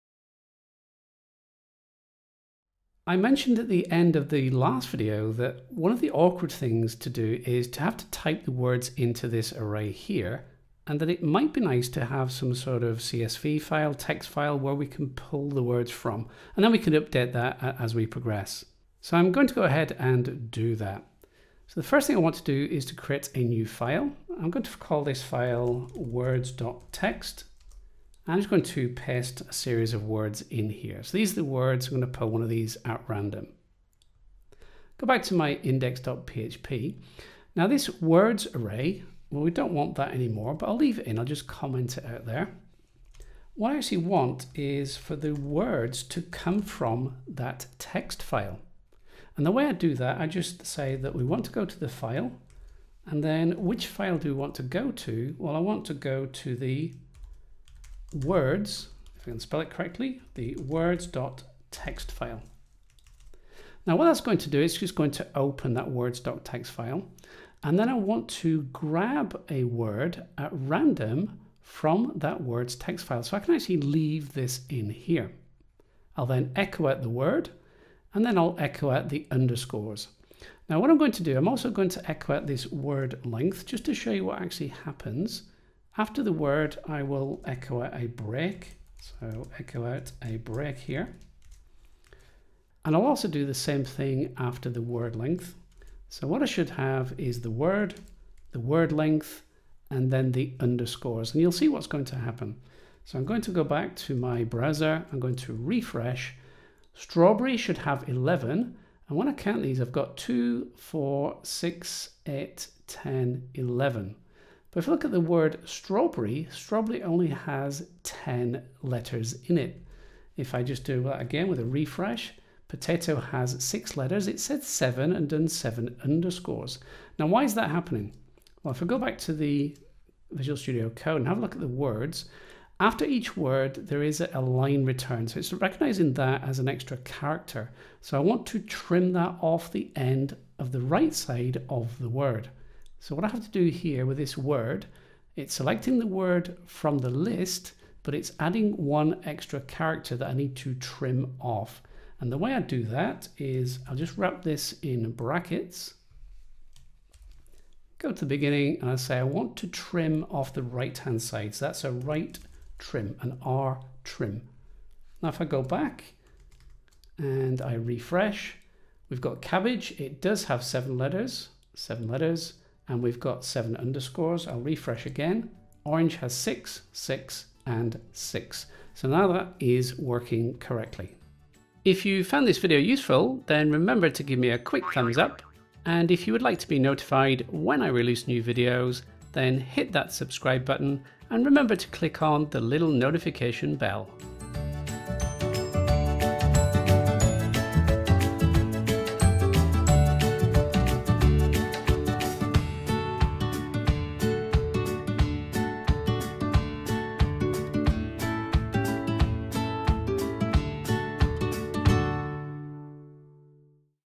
How to Build a Game using PHP - Lesson 5
Learn how to build a hangman style word game using PHP, HTML, CSS, Bootstrap, jQuery, and Font Awesome.

The full course contains over 4 hours of video, code downloads, challenges with full video solutions, and access to the teacher.

In this lesson you will learn how to read words from a text file and then place them into an array.  You will then select a word at random from the array.